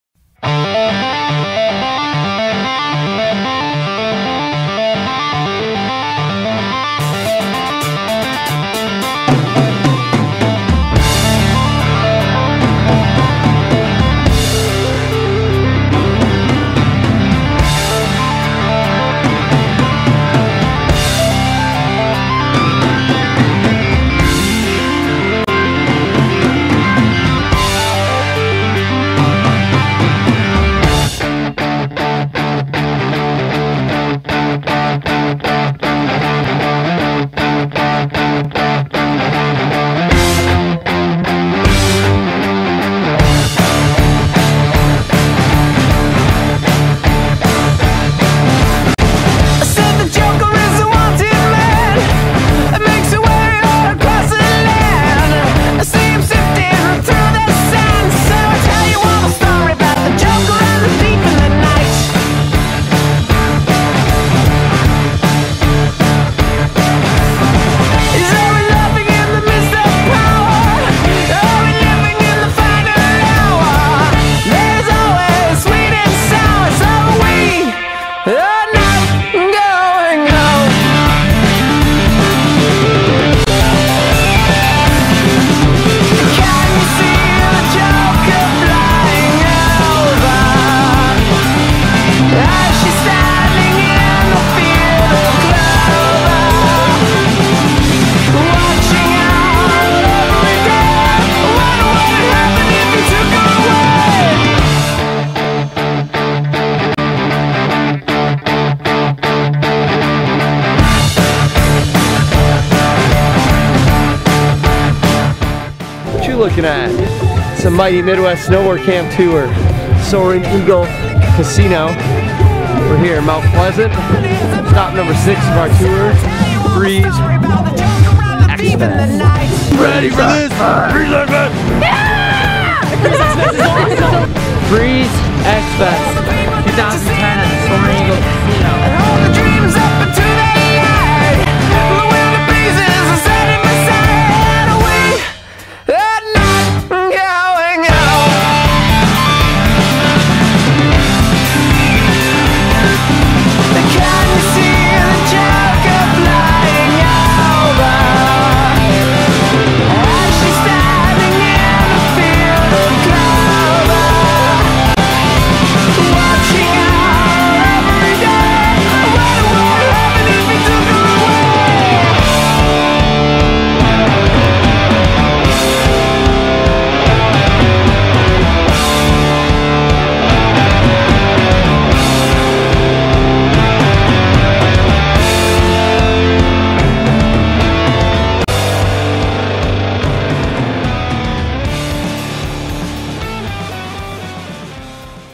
Freeze X Fest 2010 - Event Recap
Freeze X Fest at Soaring Eagle Casino is a Winter Action Sports Festival featuring the AMA Racing Ice Race Grand Championships, Mighty Midwest Snowboard Camp, USASA Rail Jam, FXF Jib Jam, Yamaha Demos, and after party.

Saturday was the one and only day that mother nature was kind to us this weekend; and although the weather didn't cooperate with us on Sunday, we had an awesome time on Saturday!  The USASA Rail Jam went off without a hitch!  Great prizes and gift certificates worth $25, $50, and $100 were given away!  On top of that, the Pat Milbery Mighty Midwest Snowboard crew threw down with some awesome teaching and sweet giveaways to all campers.  The AMA Ice Racing Ice Race Grand Championships were awesome.  Everyone loved the track and racing was spectacular!  Jimmy Blaze did a backflip, and Soaring Eagle provided us with all the great amendities including great food, a great atmosphere, and a great place to play and relax after a competitive day.